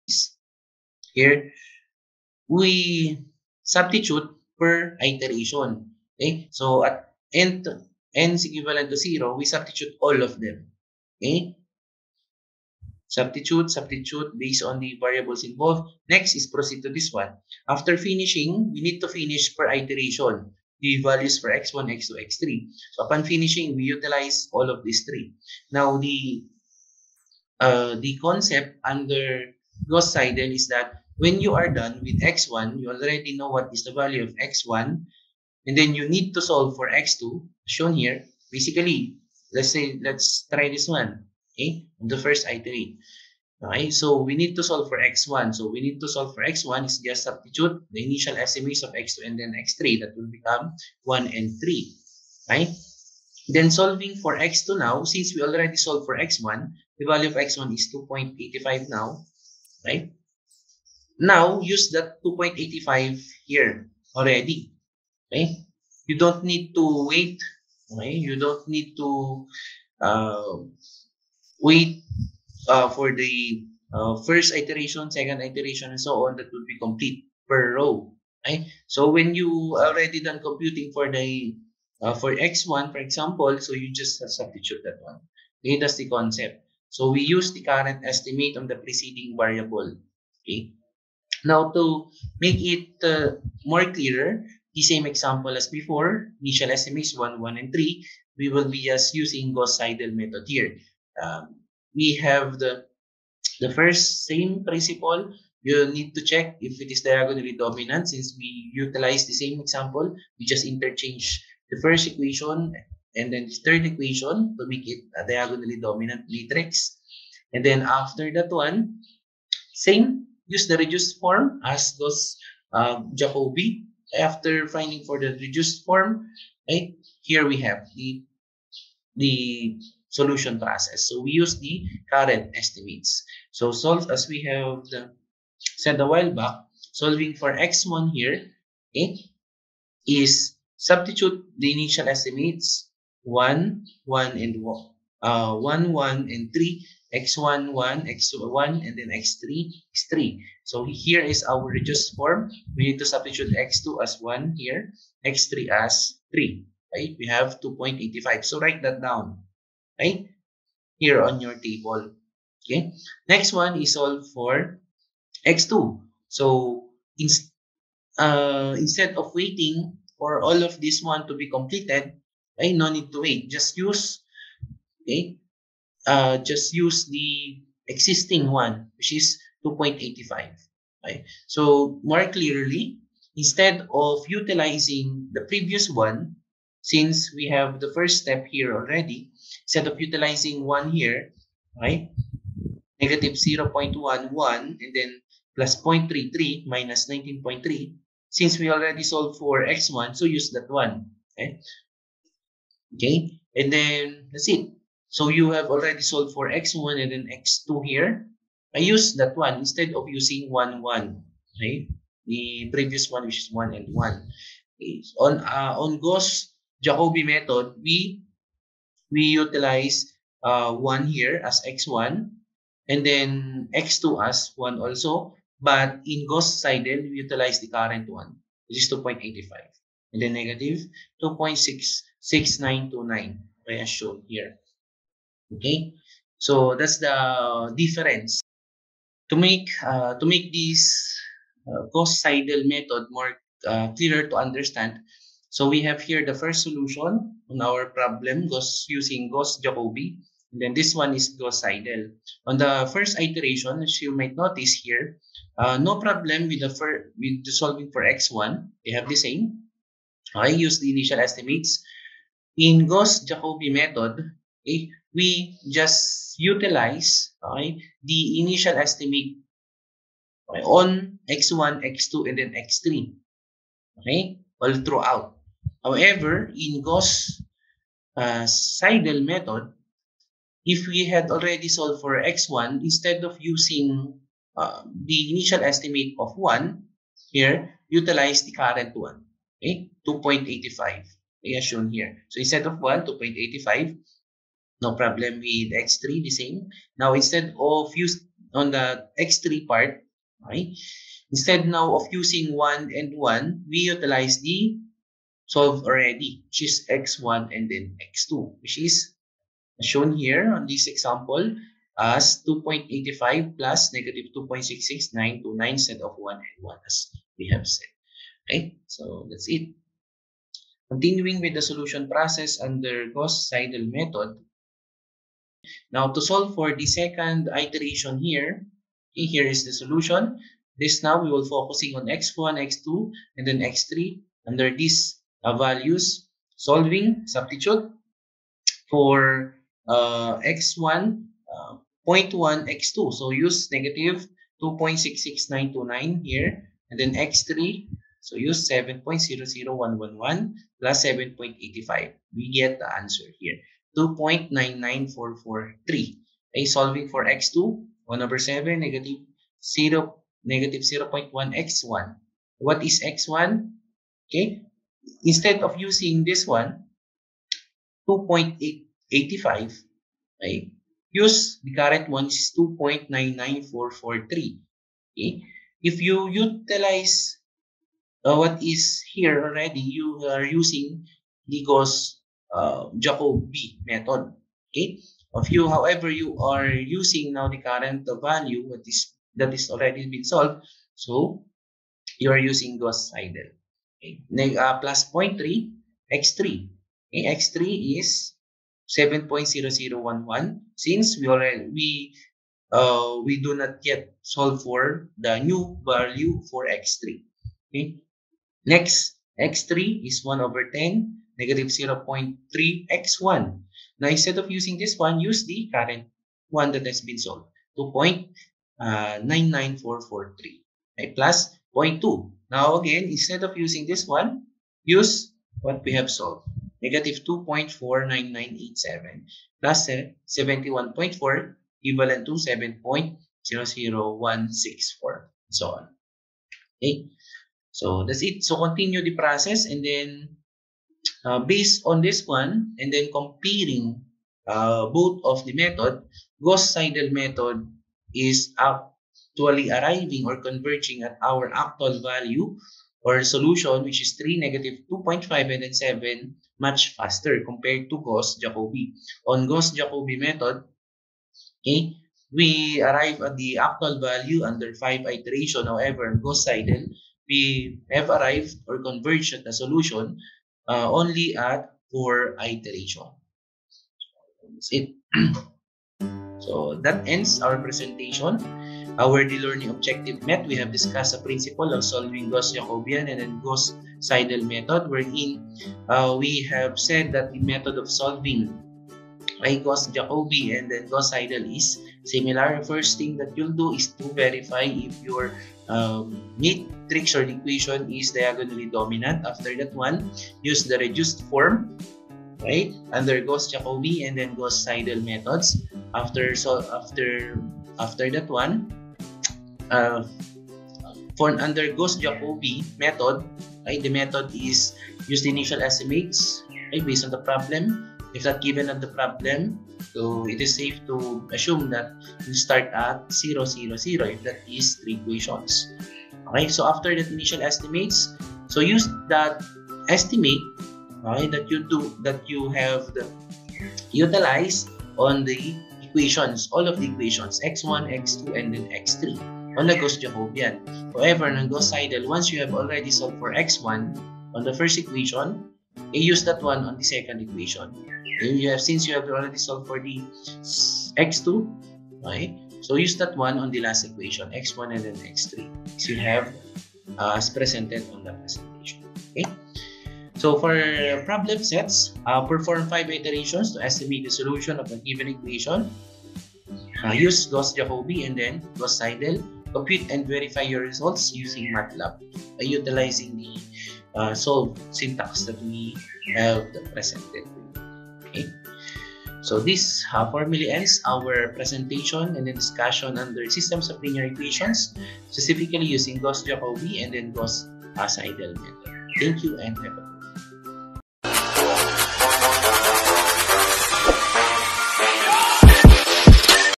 Gauss – Jacobian and Gauss – Seidel Methods 003
TOPICS: Gauss – Jacobian & Gauss – Seidel method

At the end of presentation, the student should be able to:
1. Discuss the principle of solving Gauss – Jacobian and Gauss – Seidel Methods
2. Solve a linear system using Gauss – Jacobian and Gauss – Seidel Methods